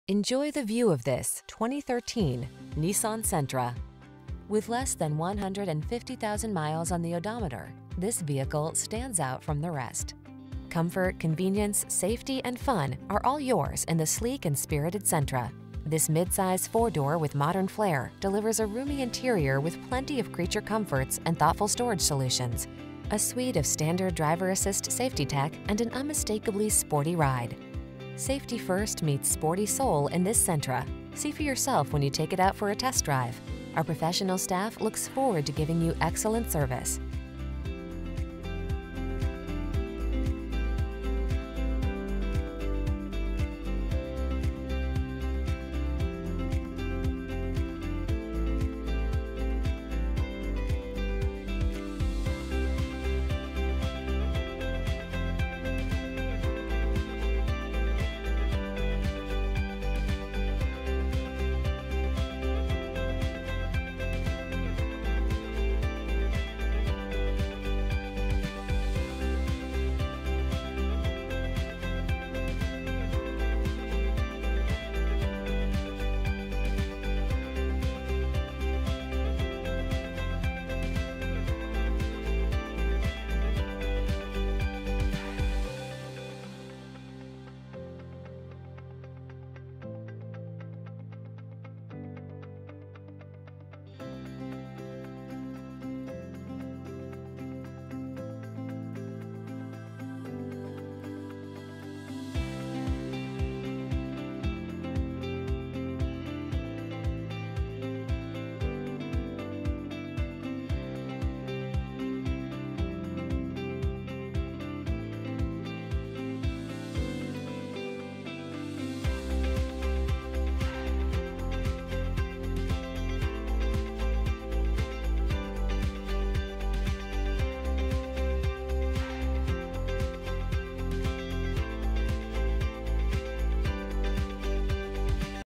2013 Nissan Sentra Redding, Eureka, Red Bluff, Northern Corning California, CA 131668
Brilliant Silver Used 2013 Nissan Sentra S available in Redding, California at Crown Motors Redding. Servicing the Eureka, Red Bluff, Northern California, Corning, CA area.
2013 Nissan Sentra S - Stock#: 131668